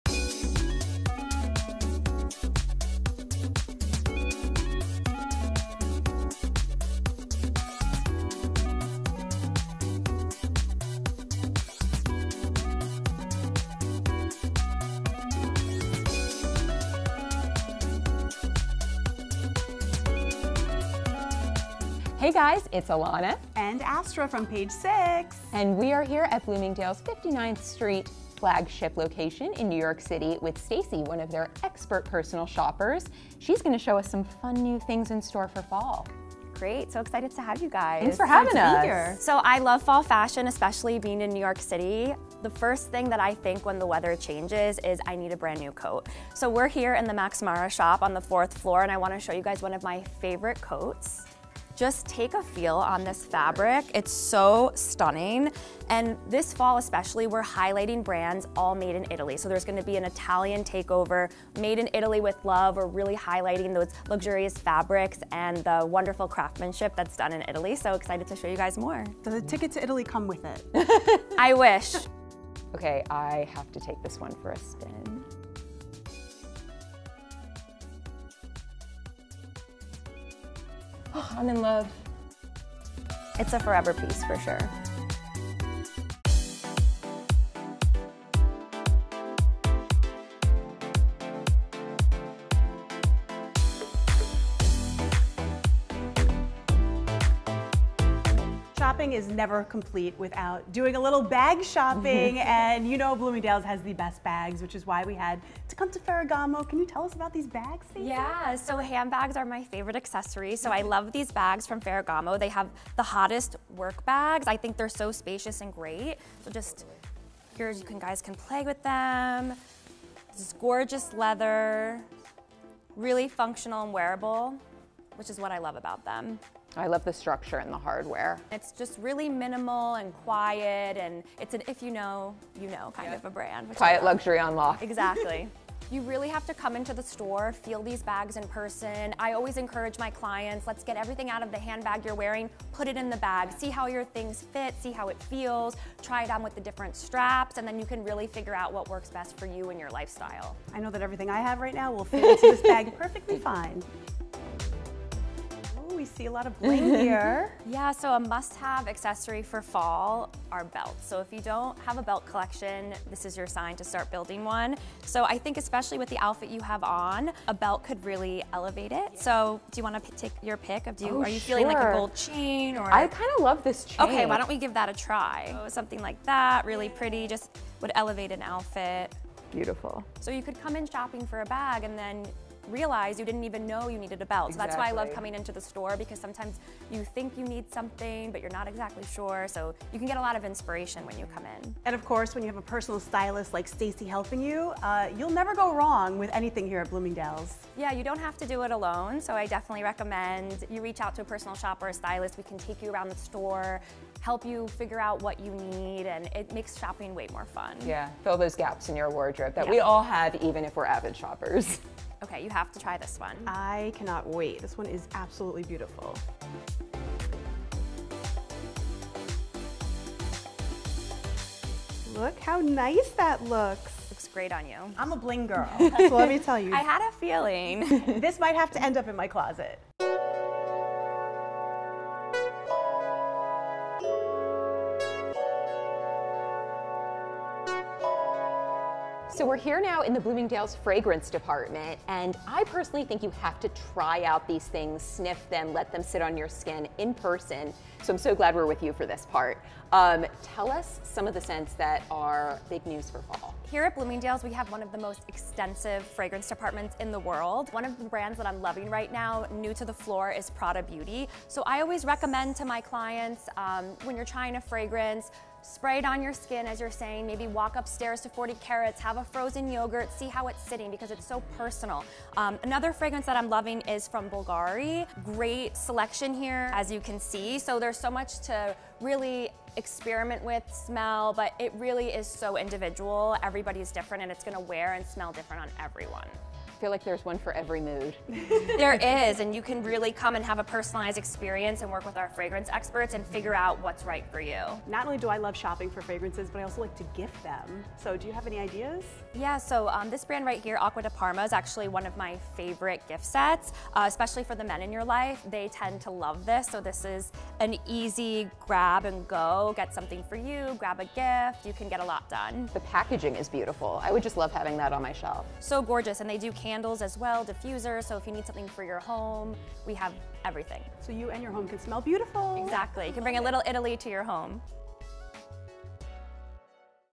Bloomingdale’s is celebrating Italy this season - let's add a little dolce vita to your wardrobe
You don’t need a round-trip ticket to Rome to add a little Italian elegance to your life.

Bloomingdale’s (@bloomingdales) recently launched “From Italy, With Love,” a two-month-long celebration of the best in Italian fashion, design, cuisine, and culture — and Page Six Style recently got up close and personal with a few of the Italian finds featured in the curation.

With Bloomingdale’s personal shopper Stacey Oliver (@ShopWithStacey) as our guide, Page Six anchor Astra and I made our way through the 59th Street flagship, stopping first at the Max Mara display, where racks of the luxury’s label’s iconic outerwear awaited.

Worn by celebrities and royals ranging from Kim Kardashian to Kate Middleton, Max Mara’s classic coats are the definition of investment pieces; crafted from the highest-quality camel hair, cashmere and wool, they’re the sort of staples you’ll wear forever.